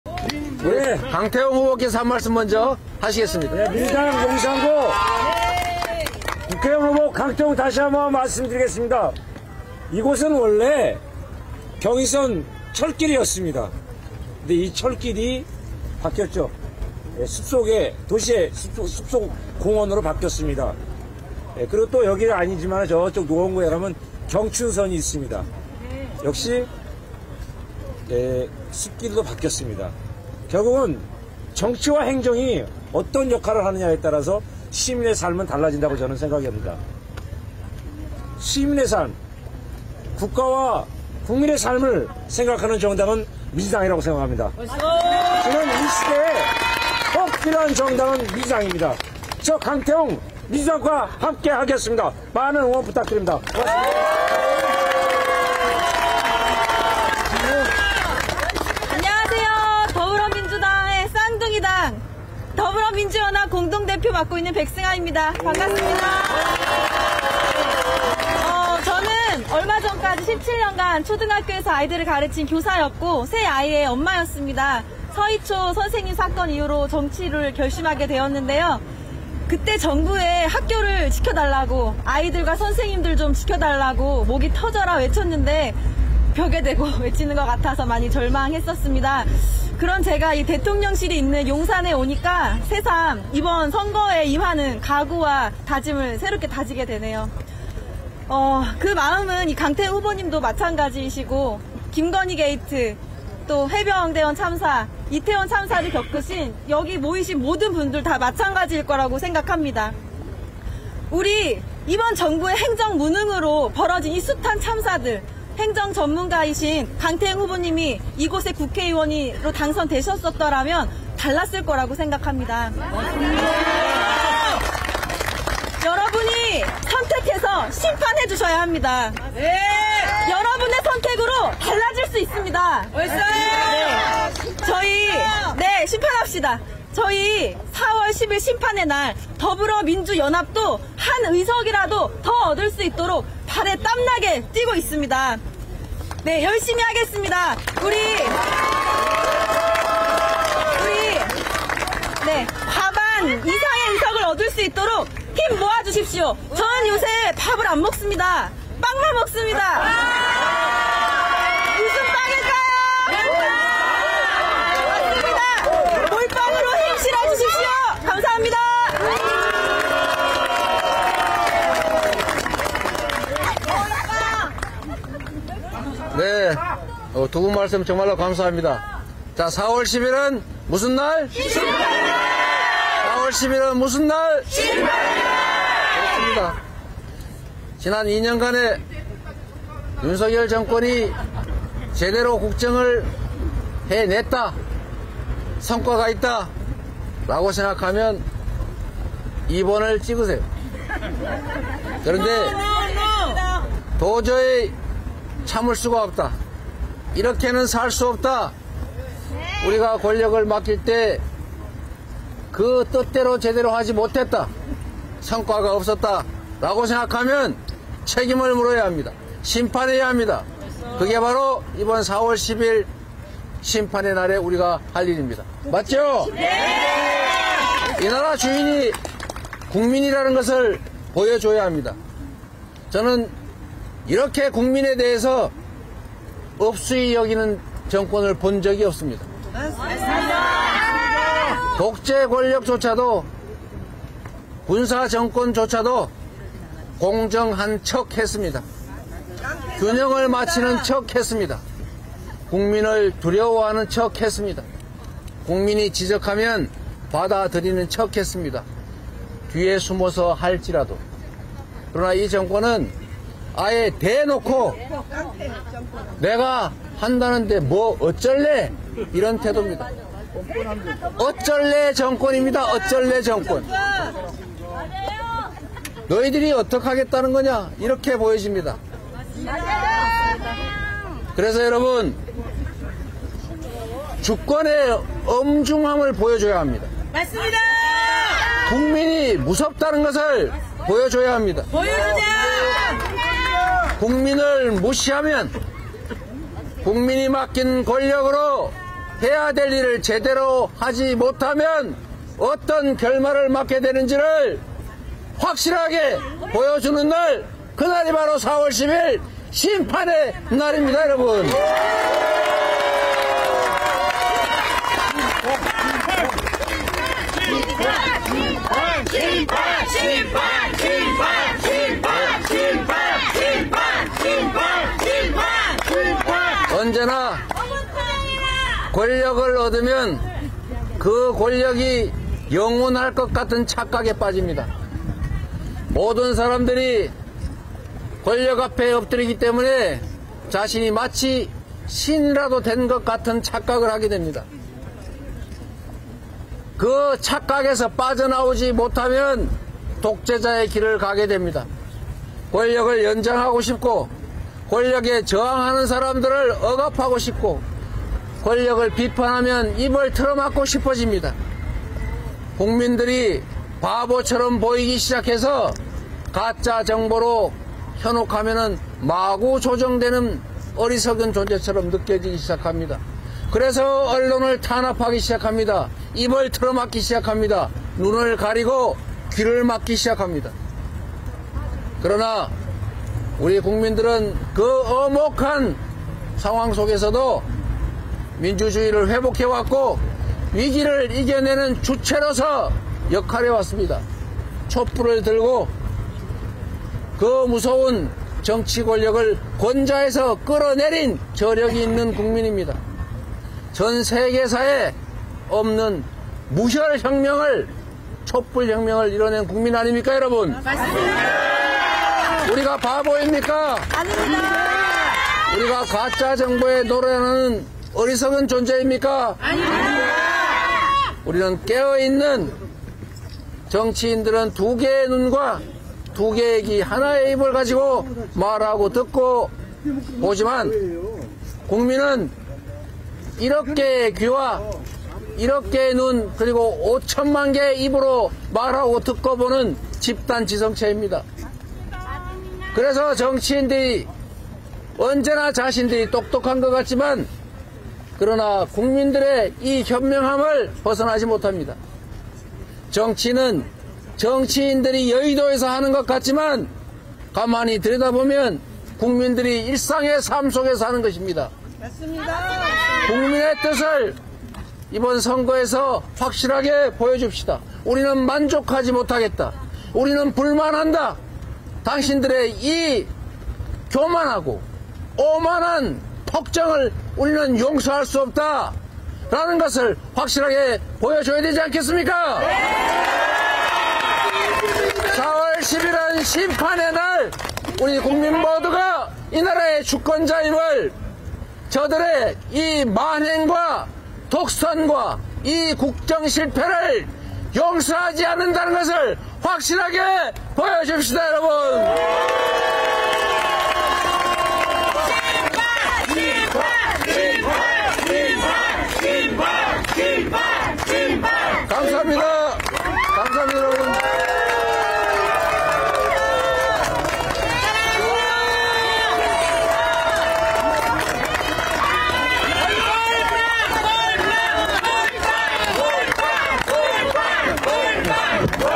대통령실 있는 용산에서 울려퍼진 “윤석열 정권 심판!” feat. ‘몰빵’ 외침과 포착된 바게트빵
이재명 더불어민주당 대표와 강태웅 후보가 13일 오후 서울 용산구 효창공원에서 기자회견을 열었다.

(영상 취재 : 권민구 기자, 김기범 PD, 박용근 PD / 영상 편집 : 강보현 PD, 김효원 PD)